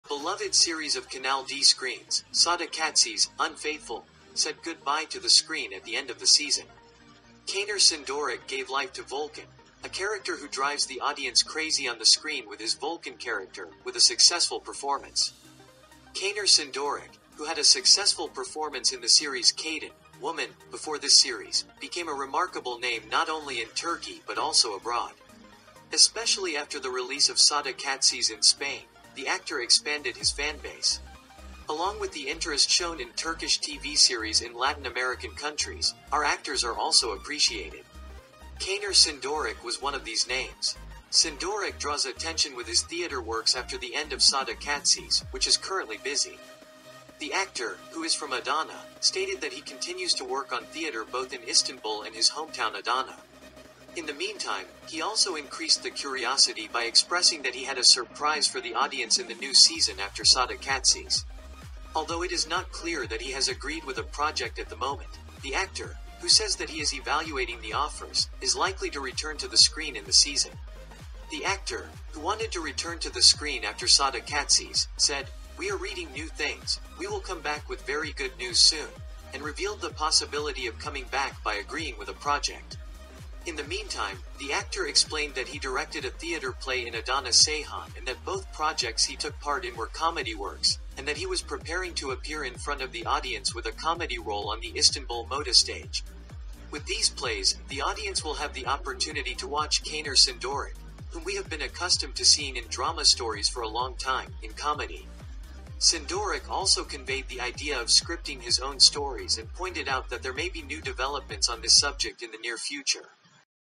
Sadakatsiz series is over, but Caner Cindoruk is preparing to surprise the audience!
The beloved series of Kanal D screens, Sadakatsiz (Unfaithful), said goodbye to the screen at the end of the season. Caner Cindoruk gave life to Volkan, a character who drives the audience crazy on the screen with his Volkan character, with a successful performance.

Caner Cindoruk, who had a successful performance in the series Kadın (Woman) before this series, became a remarkable name not only in Turkey but also abroad. Especially after the release of Sadakatsiz in Spain, the actor expanded his fan base.

Along with the interest shown in Turkish TV series in Latin American countries, our actors are also appreciated. Caner Cindoruk was one of these names. Cindoruk draws attention with his theater works after the end of Sadakatsiz, which is currently busy.

The actor, who is from Adana, stated that he continues to work on theater both in Istanbul and his hometown Adana. In the meantime, he also increased the curiosity by expressing that he had a surprise for the audience in the new season after Sadakatsiz. Although it is not clear that he has agreed with a project at the moment, the actor, who says that he is evaluating the offers, is likely to return to the screen in the season.

The actor, who wanted to return to the screen after Sadakatsiz, said, “We are reading new things, we will come back with very good news soon,” and revealed the possibility of coming back by agreeing with a project!